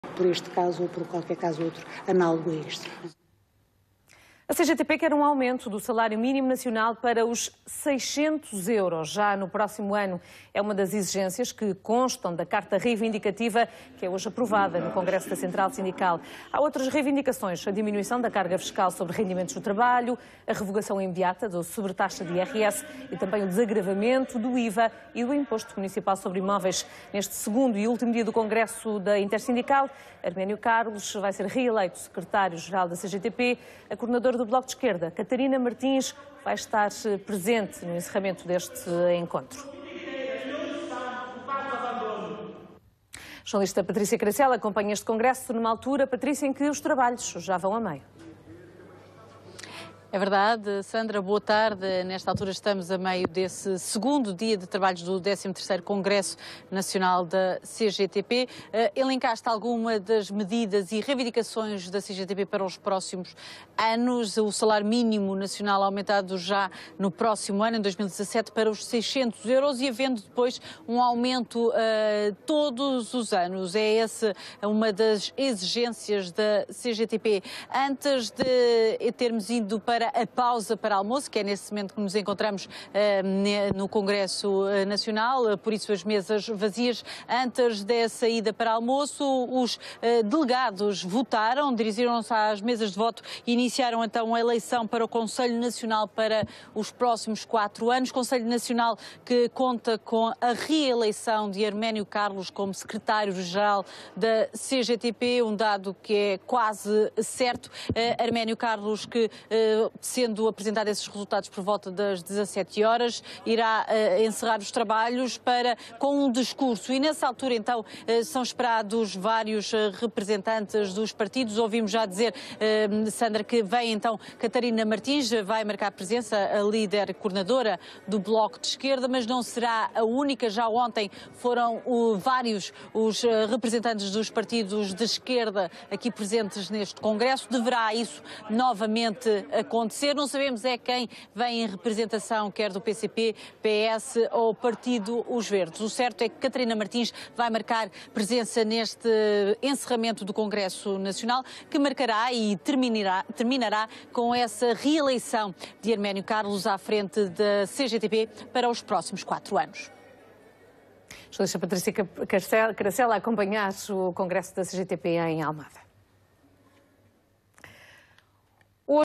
Congresso CGTP-IN - directo RTP1
A CGTP-IN quer um aumento do salário mínimo nacional para os 600 euros já no próximo ano. É uma das exigências que constam da carta reivindicativa que é hoje aprovada no congresso da central sindical.
Directo do Congresso da CGTP-IN, em Almada.